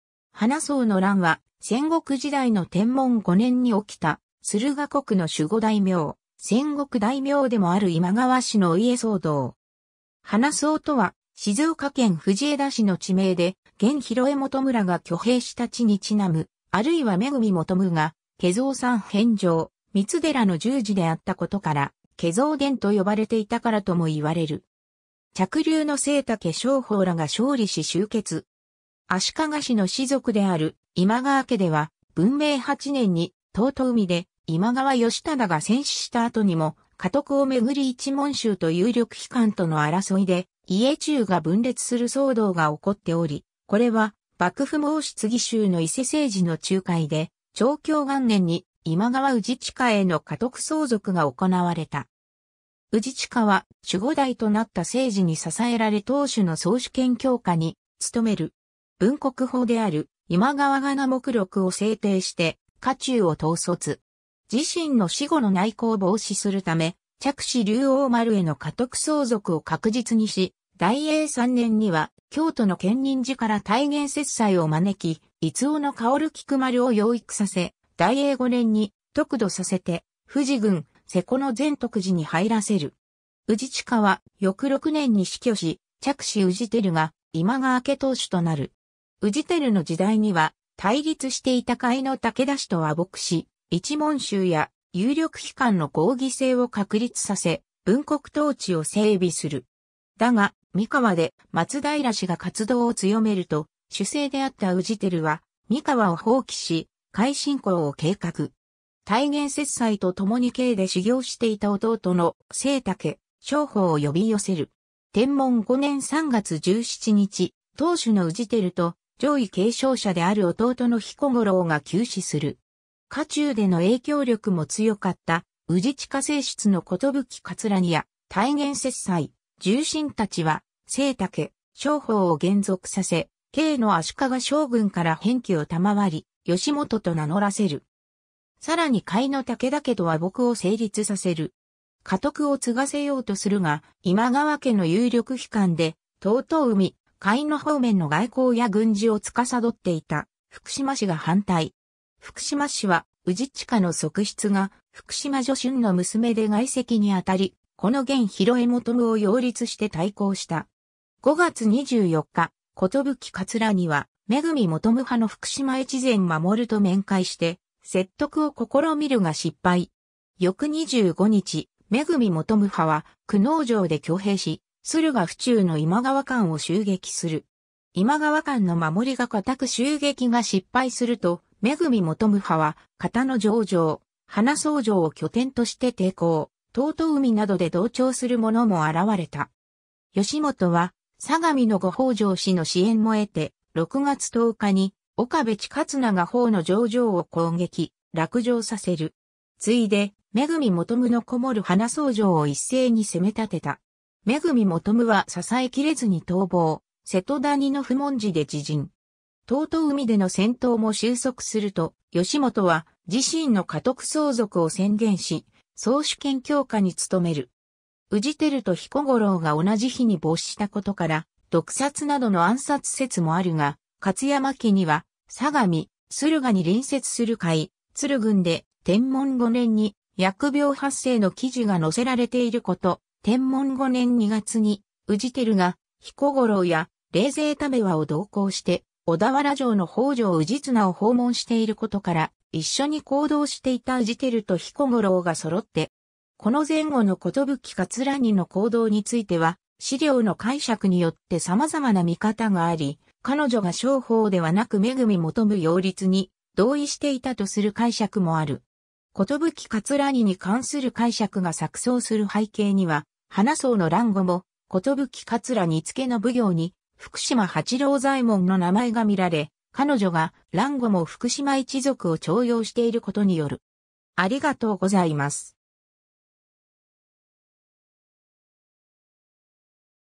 花倉の乱
花倉の乱（はなくらのらん、花蔵、はなぐらとも）は、戦国時代の天文5年（1536年）に起きた、駿河国の守護大名、戦国大名でもある今川氏のお家騒動。
「花倉」とは、静岡県藤枝市の地名で、玄広恵探らが挙兵した地にちなむ、あるいは恵探が華蔵山遍照光寺の住持であったことから「華蔵殿」と呼ばれていたからとも云われる。
嫡流の栴岳承芳（後の今川義元）らが勝利し終結。
 足利氏の氏族である今川家では、文明8年（1476年）に遠江で今川義忠が戦死した後にも、家督を巡り一門衆と有力被官との争いで家中が分裂する騒動が起こっており、これは幕府申次衆の伊勢盛時（北条早雲）の仲介で長享元年（1487年）に今川氏親への家督相続が行われた。
 氏親は、守護代となった盛時に支えられ当主の宗主権強化に努める。
分国法である『今川仮名目録』を制定して家中を統率。
自身の死後の内訌を防止するため、嫡子龍王丸（今川氏輝）への家督相続を確実にし、大永3年（1523年）には京都の建仁寺から太原雪斎を招き、五男の芳菊丸（正室寿桂尼の第三子。のちの栴岳承芳、義元）を養育させ、大永5年（1525年）に得度させて富士郡瀬古の善得寺（静岡県富士市）に入らせる。
氏親は翌6年（1526年）に死去し、嫡子氏輝が今川家当主となる。
 氏輝の時代には、対立していた甲斐の武田氏と和睦し、一門衆や有力被官の合議制を確立させ、分国統治を整備する。
だが、三河で松平氏が活動を強めると、守勢であった氏輝は三河を放棄し、甲斐侵攻を計画。
太原雪斎とともに京で修行していた弟の栴岳承芳（義元）を呼び寄せる。
 天文5年3月17日（1536年4月7日）、当主の氏輝と、上位継承者である弟の彦五郎が急死する。
家中での影響力も強かった氏親正室の寿桂尼や、太原雪斎、重臣たちは栴岳承芳（義元）を還俗させ、京の足利将軍から偏諱を賜り、義元と名乗らせる。
さらに甲斐の武田家と和睦を成立させる。
家督を継がせようとするが、今川家の有力被官で、遠江、甲斐方面の外交や軍事を司っていた福島氏が反対。
福島氏は氏親の側室が福島助春の娘で外戚にあたり、子の玄広恵探を擁立して対抗した。
 5月24日（6月12日）、寿桂尼は恵探派の福島越前守（福島正成と同一人物説がある）と面会して説得を試みるが失敗。
翌25日（13日）、恵探派は久能城で挙兵し、駿河府中の今川館を襲撃する。
今川館の守りが堅く襲撃が失敗すると、恵探派は方ノ上城（焼津市）、花倉城（葉梨城、藤枝市）を拠点として抵抗、遠江などで同調するものも現れた。
 義元は相模の後北条氏の支援も得て、6月10日（6月28日）に岡部親綱が方ノ上城を攻撃、落城させる。
次いで恵探の篭る花倉城をいっせいに攻め立てた。
恵探は支えきれずに逃亡、瀬戸谷の普門寺で自刃。
遠江での戦闘も収束すると、義元は自身の家督相続を宣言し、宗主権強化に努める。
 ^ 氏輝と彦五郎が同じ日に没したことから、毒殺などの暗殺説もあるが、『勝山記』には相模・駿河に隣接する甲斐・都留郡で天文5年に疫病発生の記事が載せられていること、天文5年2月に氏輝が彦五郎や冷泉為和を同行して小田原城の北条氏綱を訪問していることから、一緒に行動していた氏輝と彦五郎が揃って疫病にかかったとしても不自然ではない、とする見方がある。
 ^ この前後の寿桂尼の行動については史料の解釈によって様々な見方があり、彼女が承芳（義元）ではなく恵探擁立に同意していたとする解釈もある。
寿桂尼に関する解釈が錯綜する背景には花倉の乱後も寿桂尼付の奉行（内衆膳方奉行）に「福島八郎左衛門」の名前が見られ、彼女が乱後も福島一族を重用していることによる。